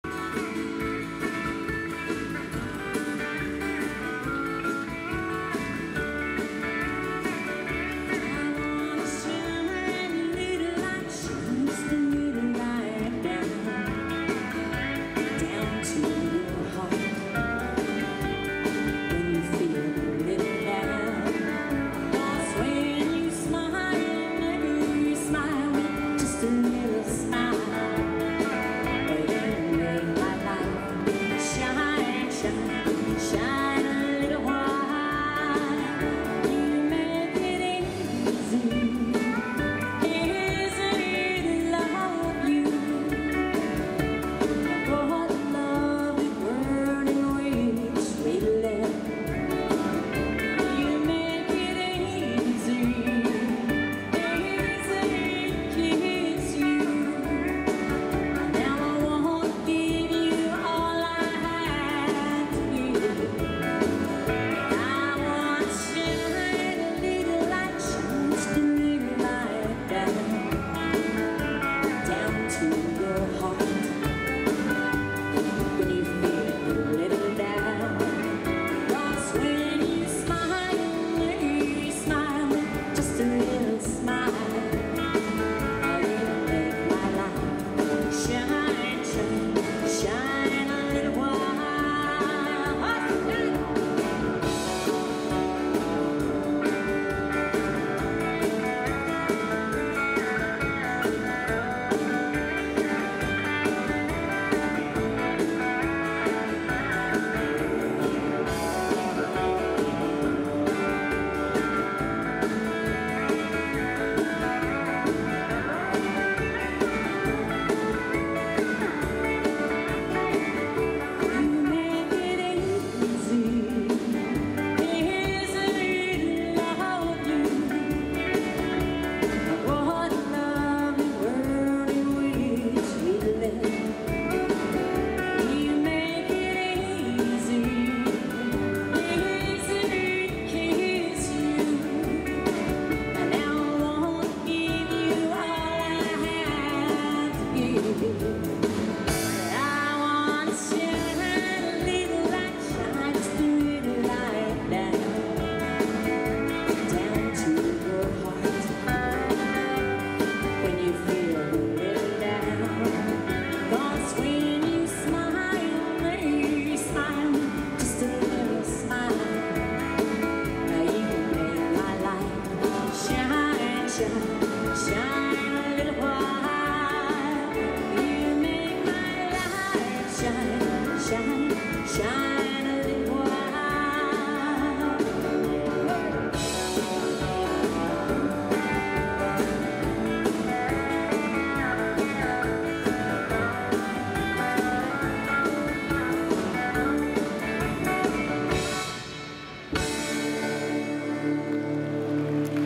kim carson
en concert à caen america normandy festival